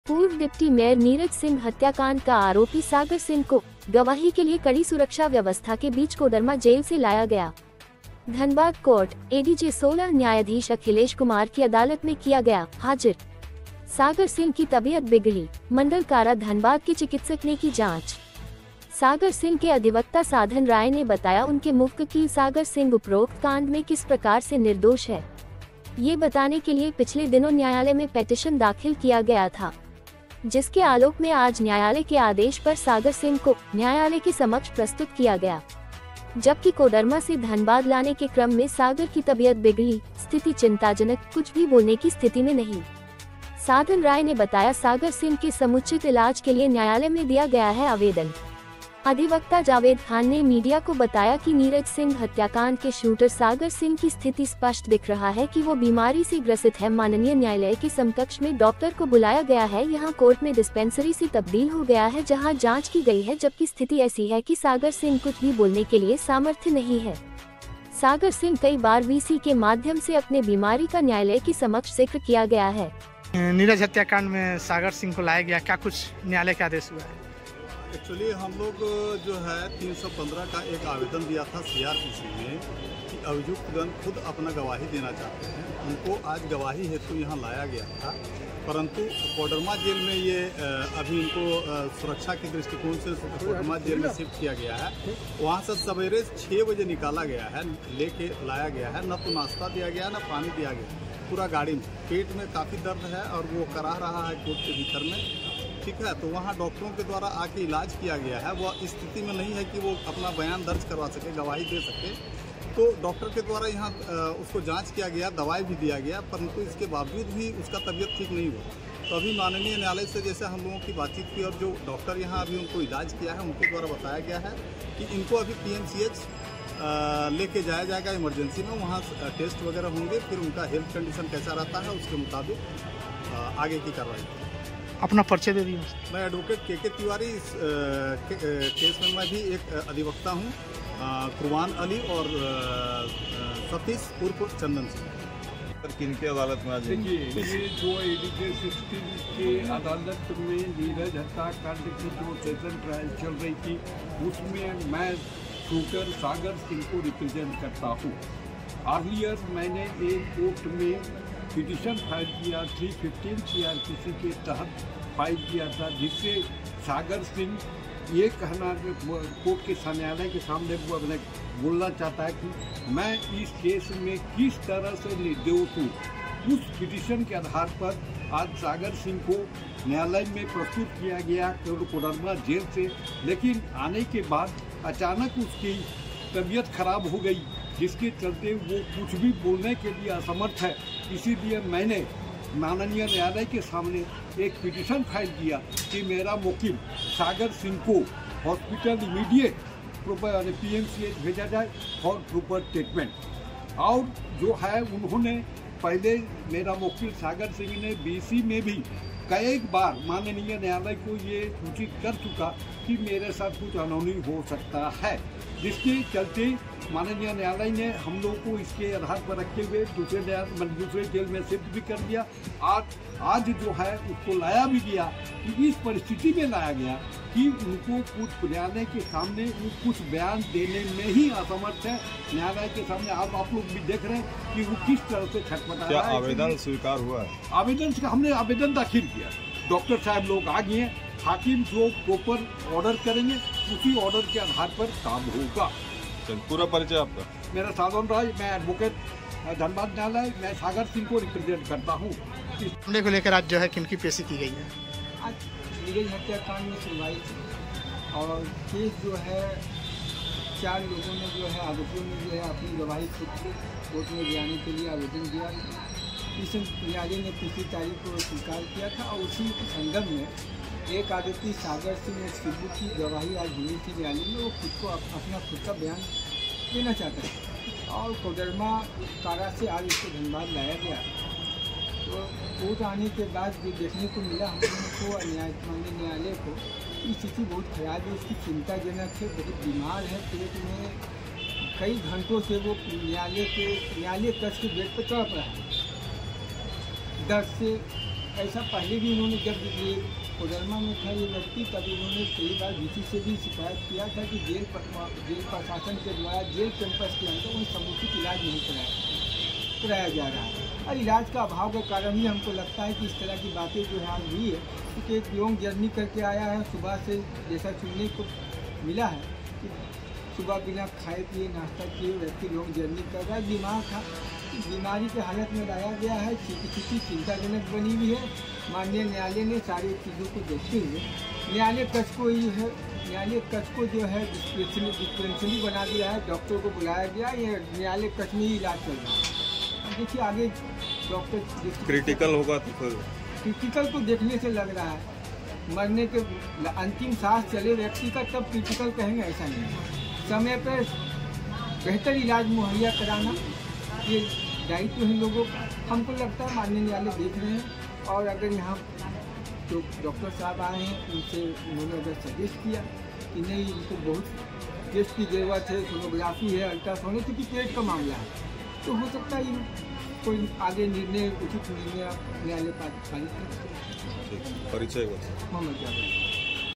नीरज सिंह हत्या कांड का को गवाही के लिए कड़ी सुरक्षा व्यवस्था के बीच कोडरमा जेल से लाया गया
नीरज सिंह हत्या कांड का आरोपी सागर सिंह को गवाही के लिए कड़ी सुरक्षा व्यवस्था के बीच कोडरमा जेल से लाया गया